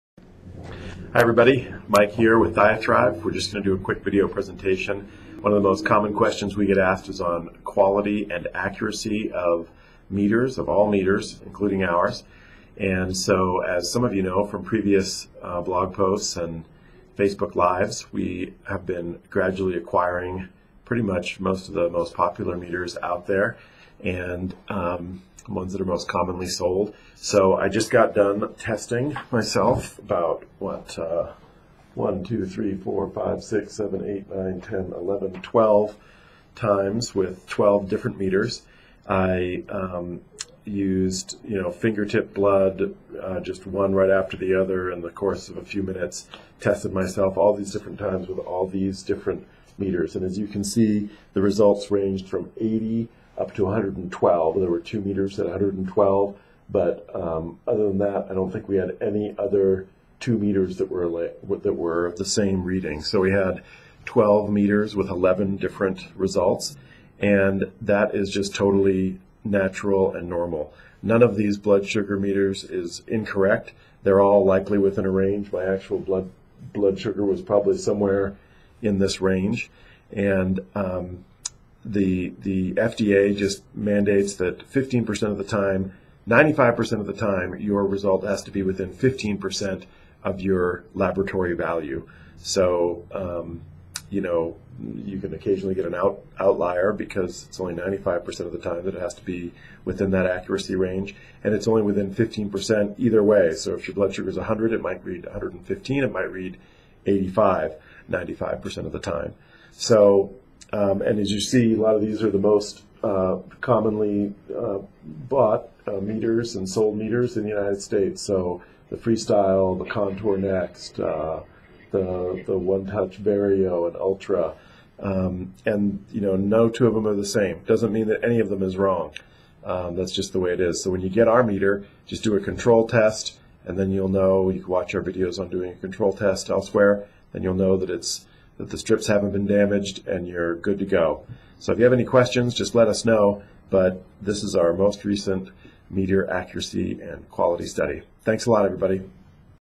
Diathrive Meter Comparison
Do you think your glucose meter is accurate?  Watch us compare 12 popular glucose meters.  for more information about diabetes testing supplies, diabetes and helpful hints for living with diabetes.